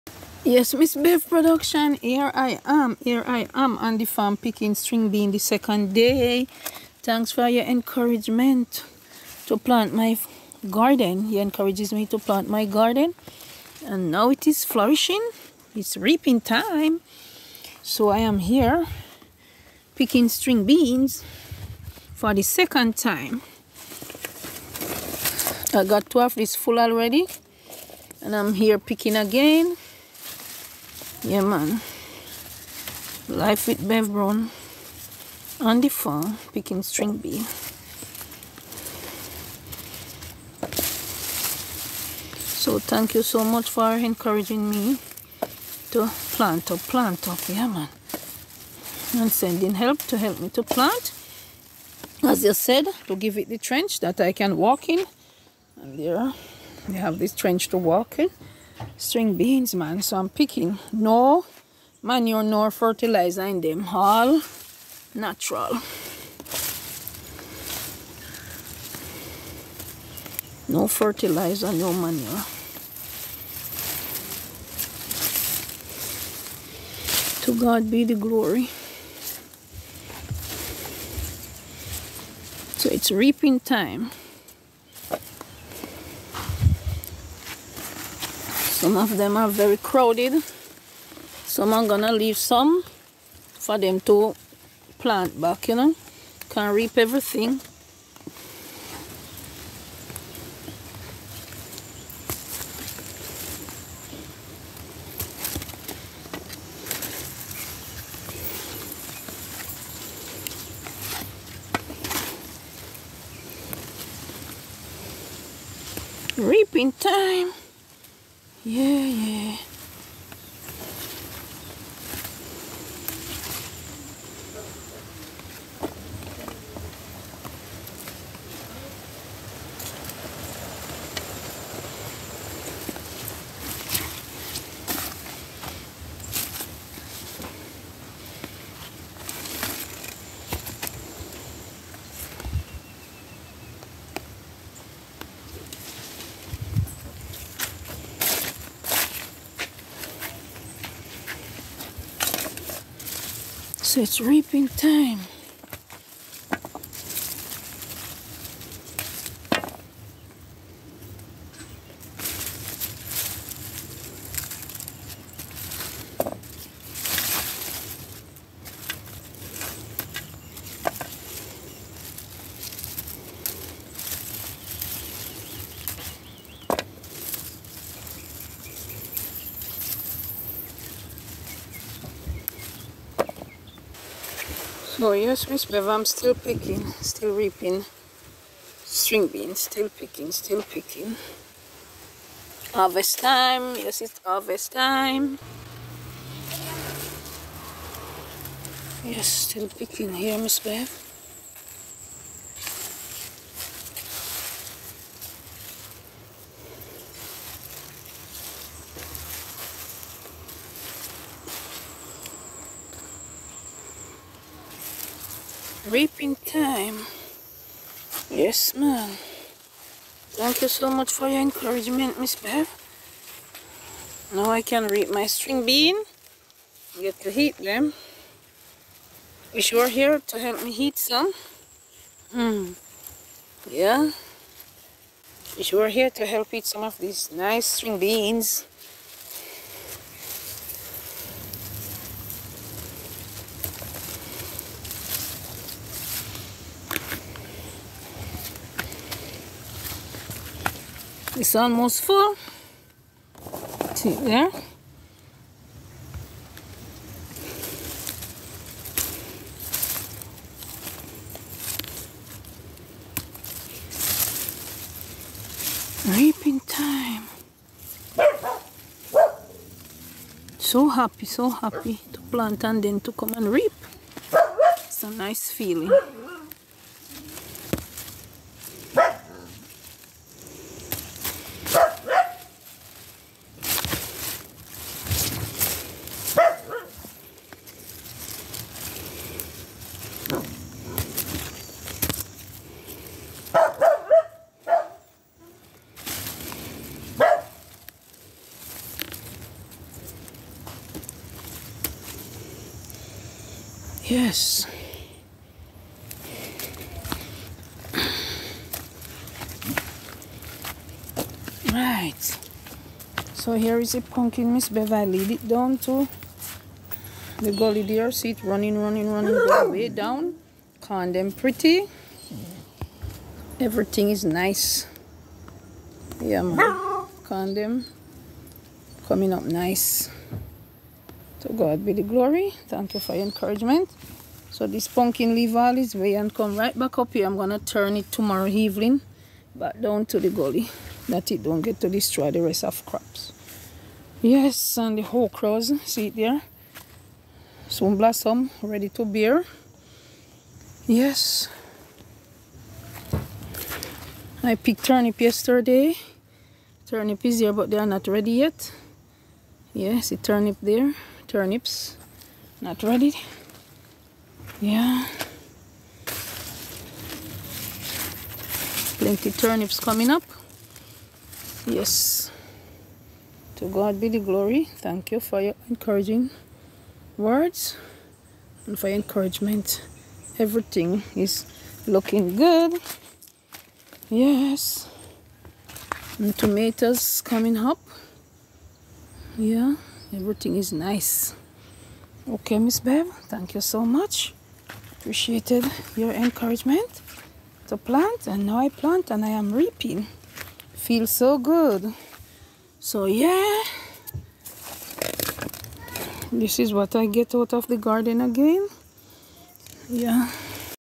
Big Up Bevbrown!!! String beans look good 👍 😋 👌 😍
MBP family, Bevbrown sent this video to share her farm produce with youuuu.  By the sweat of our brows, we truly will eat bread